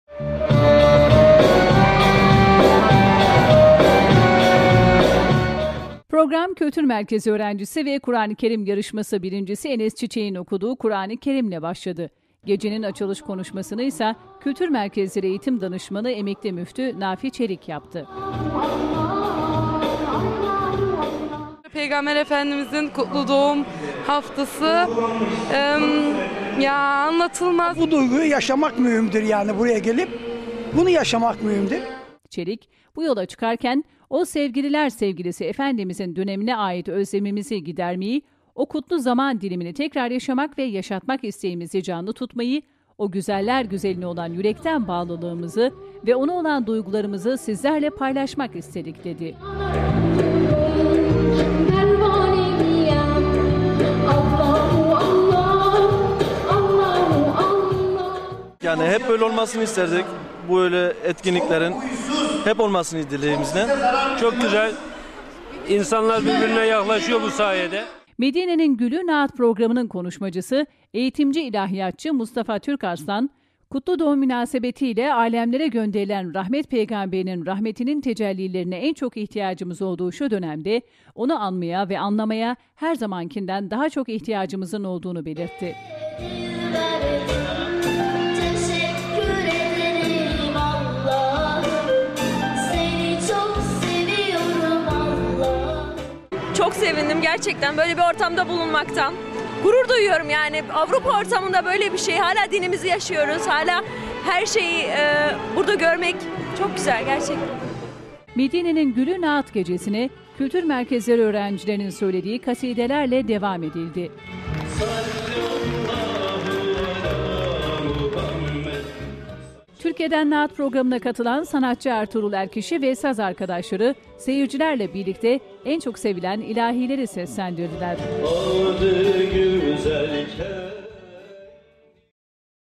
MÜLHEİM'DA MEDİNE'NİN GÜLÜ NAAT GECESİ
GECEYE ÇOK SAYIDA PEYGAMBER SEVDALISI KATILDI